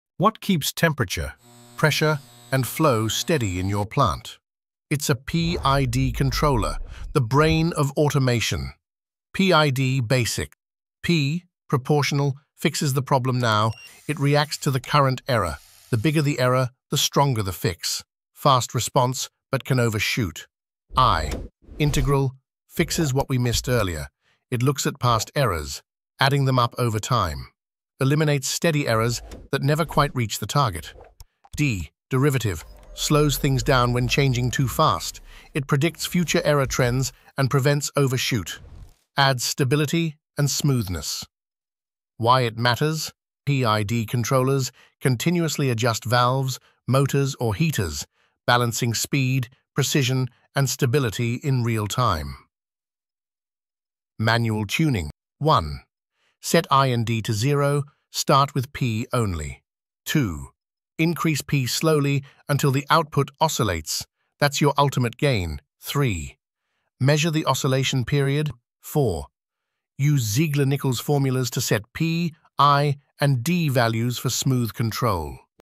PID Controller & Tuning Explained in 1 Minute - Easy & Fast!
PID Controller & Tuning Explained in 1 Minute!

In this quick video, you’ll learn the basics of a PID Controller — the brain behind many industrial automation systems. We break down the three key parts:

P (Proportional): Reacts to current error

I (Integral): Fixes past accumulated errors

D (Derivative): Predicts future errors to reduce overshoot

Plus, get simple tips on how to tune these settings for smooth and stable control.

Whether you're new to automation or just need a quick refresher, this video makes PID controllers easy to understand — fast!